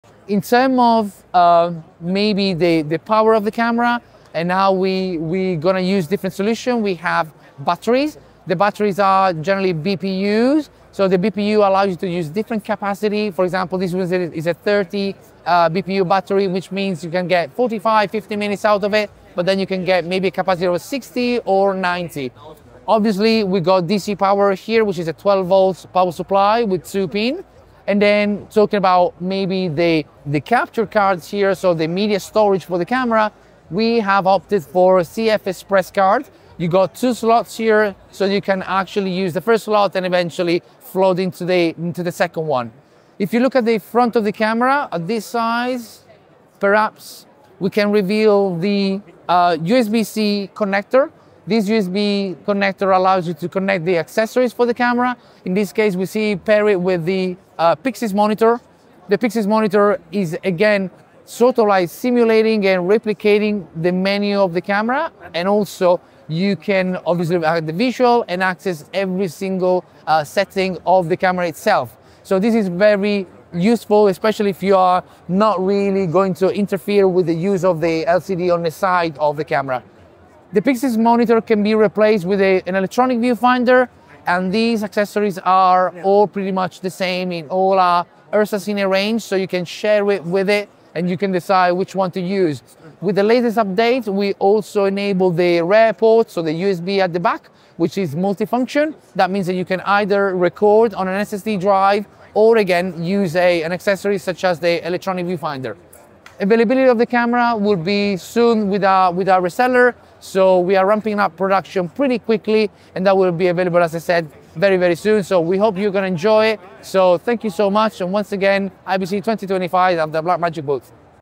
Blackmagic представляє PYXIS 12K - потужну та зручну 12K камеру IBC2025 dreamtechua￼
На IBC 2025 ми представляємо нову камеру PYXIS 12K від Blackmagic. Ця камера поєднує передові технології та зручність використання. Вона має повнокадровий RGBW-сенсор 12K, компактний корпус та широкі можливості підключення для швидкого вивантаження медіа у пост-продакшн, використання різних рішень, акумуляторів, карт пам'яті CFexpress, USB-C роз'єм для підключення аксесуарів, такий як монітор Pixis, який дозволяє візуально керувати налаштуваннями камери. Також згадуємо про задній багатофункціональний USB порт та скорий вихід камери у продаж через наших реселерів.

￼ IBC2025 dreamtechua￼ Blackmagic PYXIS12K SensorTechnology FullFrame RGBW Compact Versatile Connectivity ￼камера акумулятор CFexpress USB-C